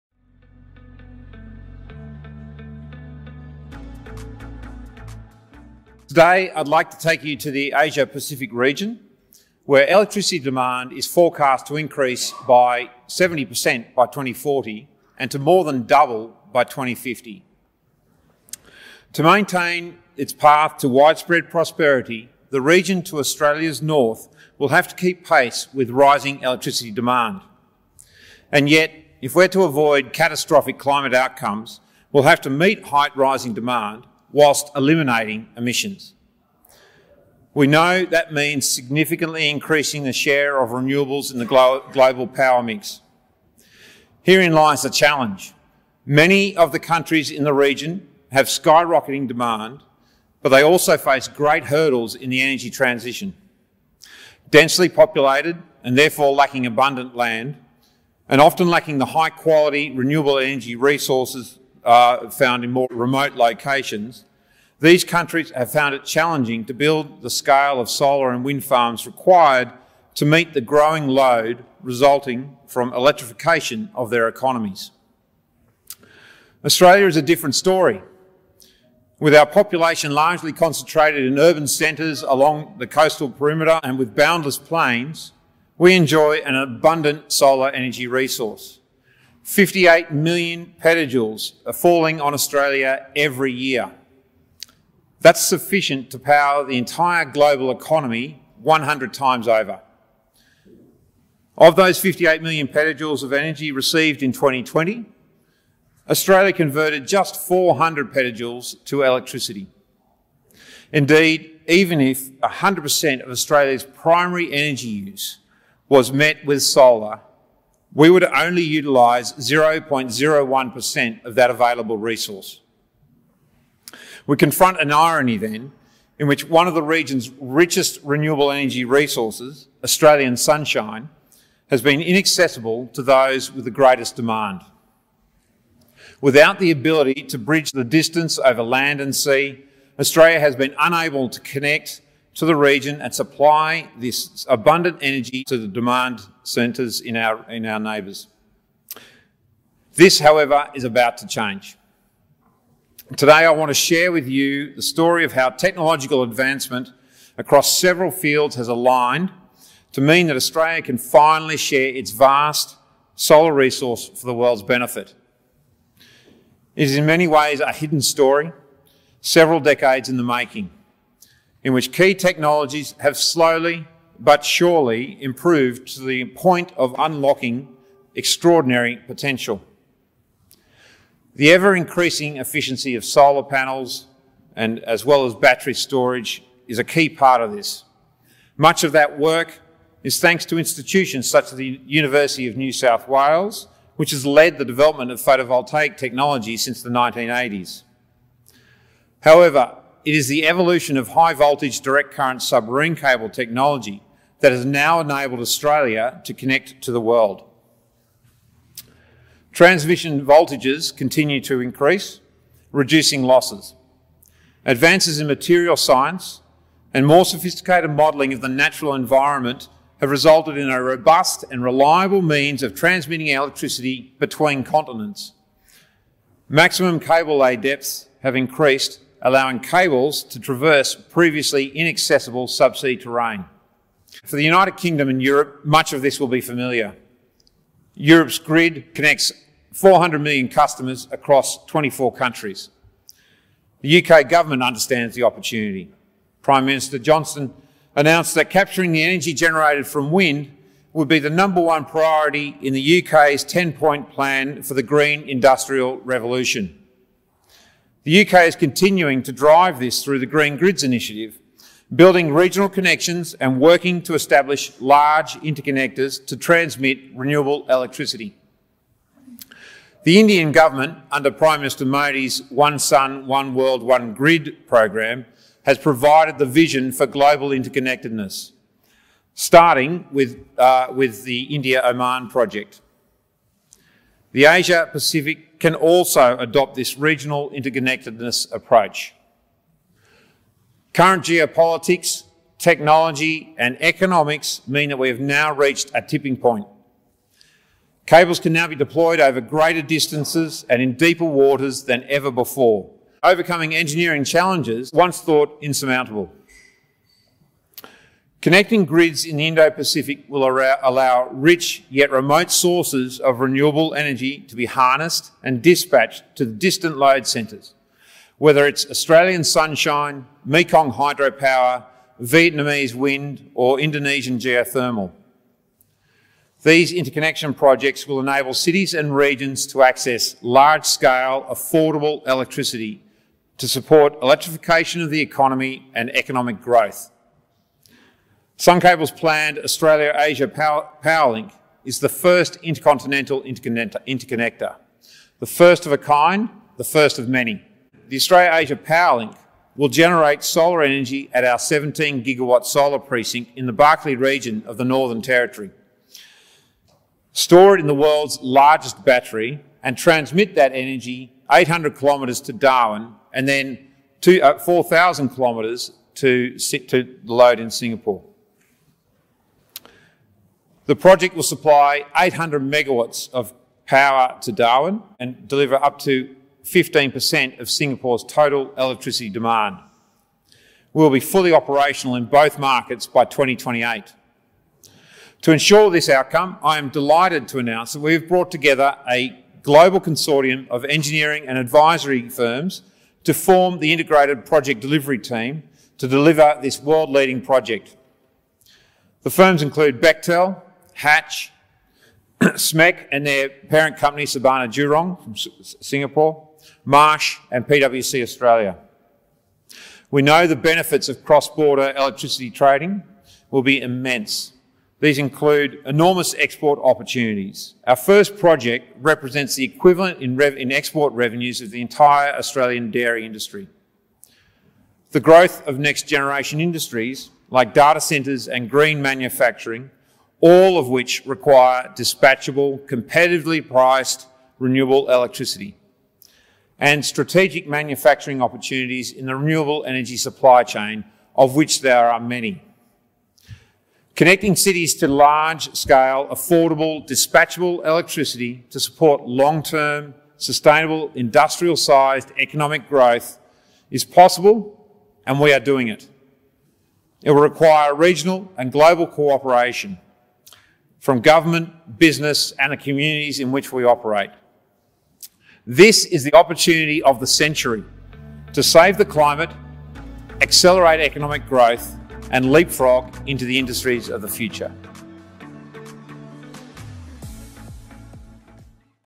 David Griffin, Sun Cable - UK/AU Climate Talks
The Australian government has set out its technology-driven plan to reach net zero.

Listen to David Griffin, CEO of Sun Cable as he announces the multi-gigawatt Australia-Asia Powerlink, a project that is designed to significantly accelerate the carbon zero ambitions of the region.